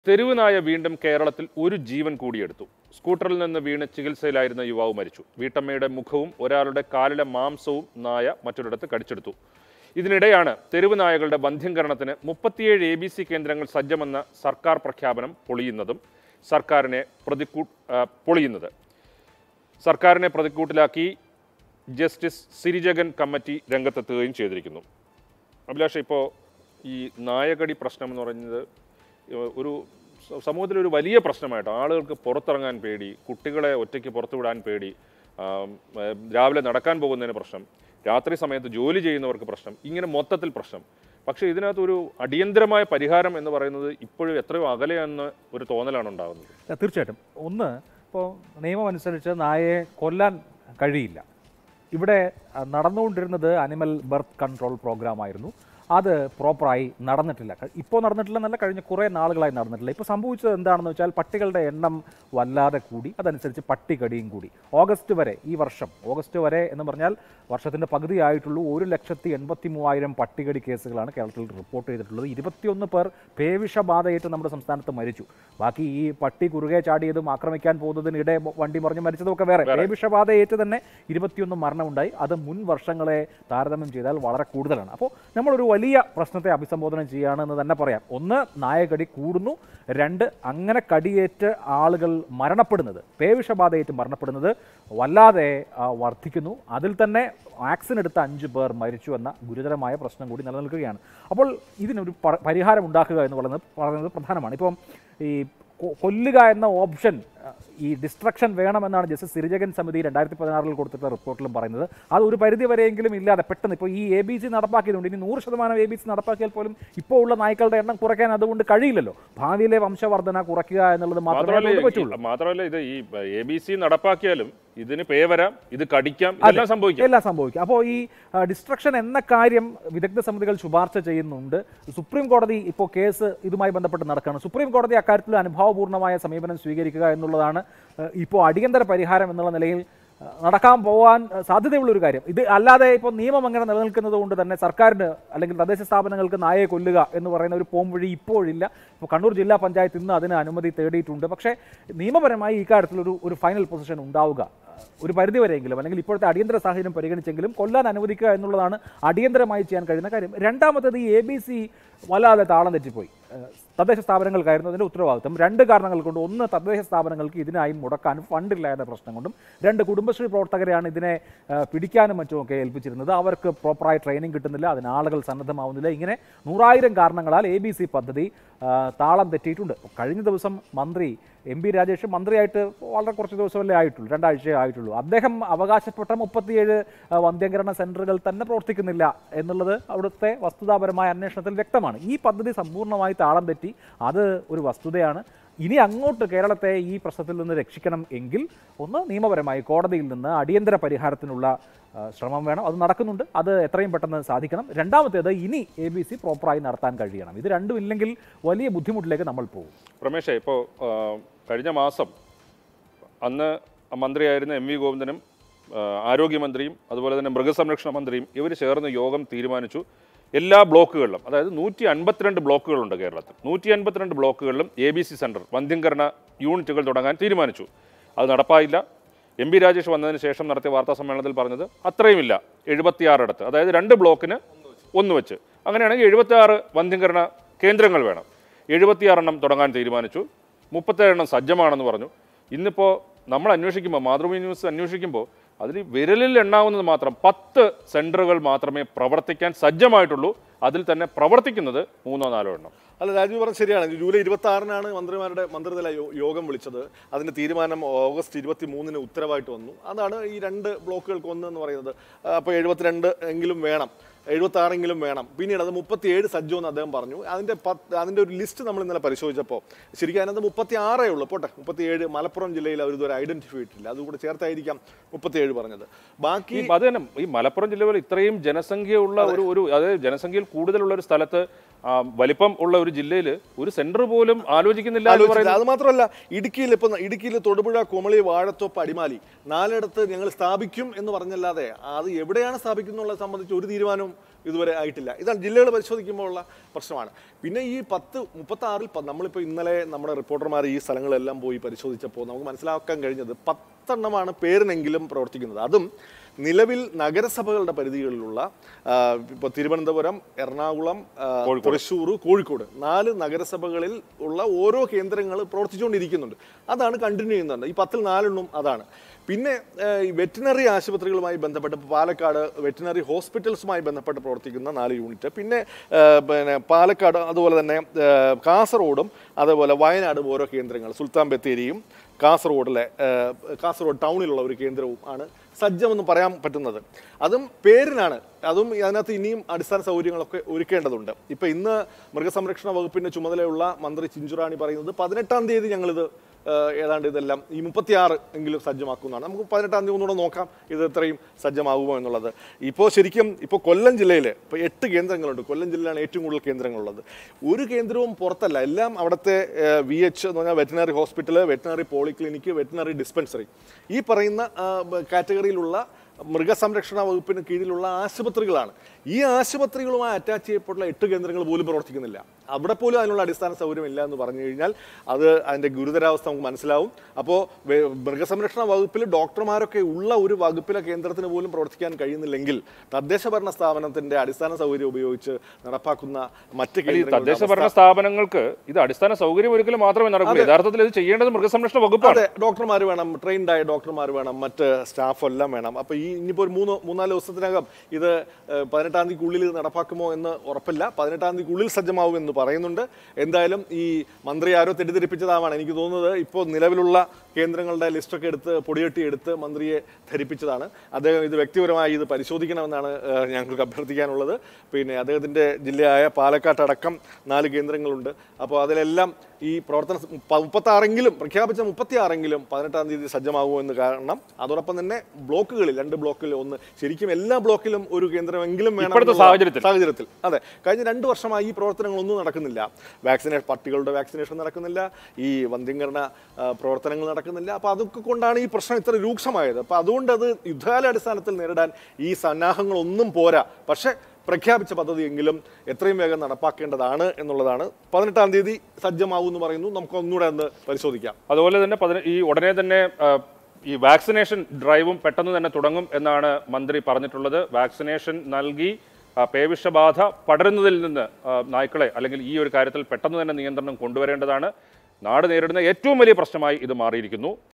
100 ശതമാനം എബിസി നടത്തിയാലും ഇപ്പോഴുള്ള നായകളുടെ എണ്ണം കുറയ്ക്കാന്‍ അത് കൊണ്ട് കഴിയില്ല |Street Dog
കേരളത്തെ തെരുവ് നായ പ്രശ്‌നത്തില്‍ നിന്നും രക്ഷിക്കണമെങ്കില്‍ കോടതിയില്‍ നിന്ന് അടിയന്തര പരിഹാരത്തിനുള്ള ശ്രമം വേണമെന്നും ഇനി എബിസി ശരിയായ രീതിയില്‍ നടത്താന്‍ കഴിയണമെന്നും അഭിലാഷ് മോഹനന്‍ ന്യൂസ് എക്‌സ്ട്രയില്‍.

Happy viewing!